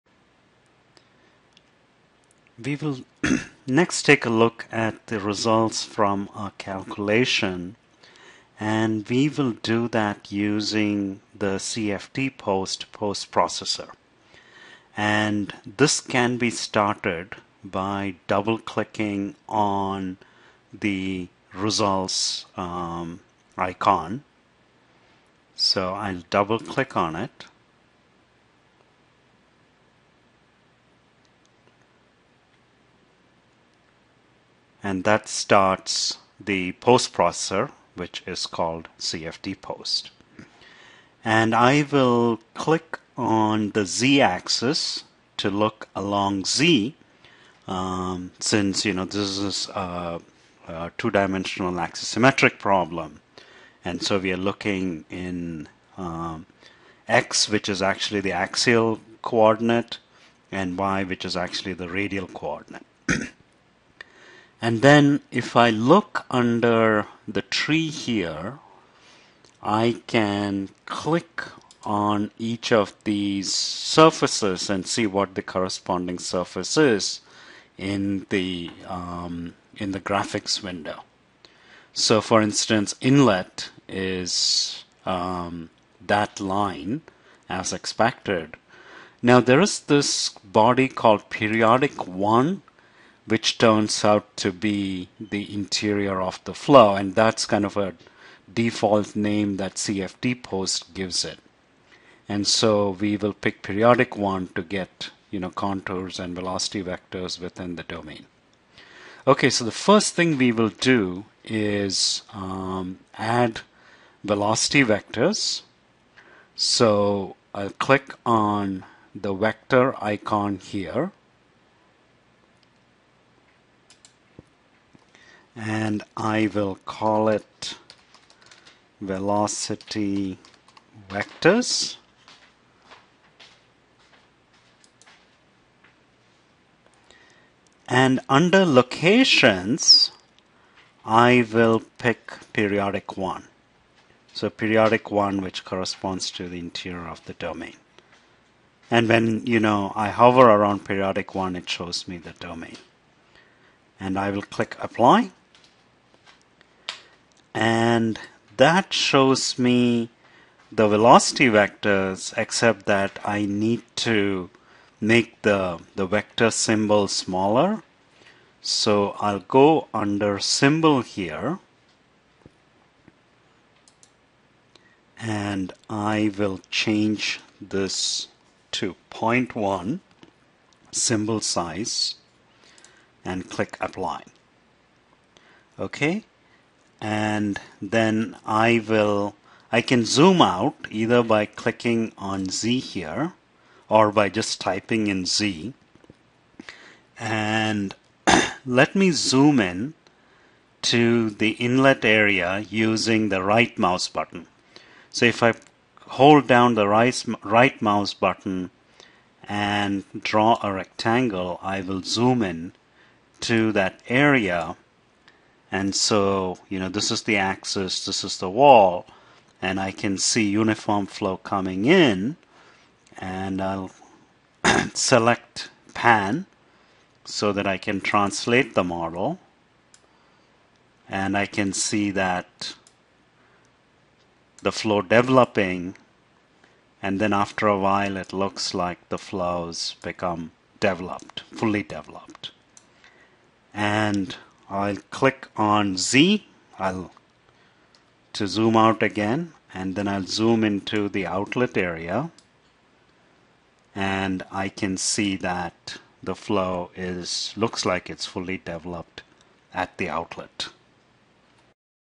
Velocity vectors for laminar pipe flow
Visualization of velocity vectors for laminar pipe flow from the following SimCafe tutorial: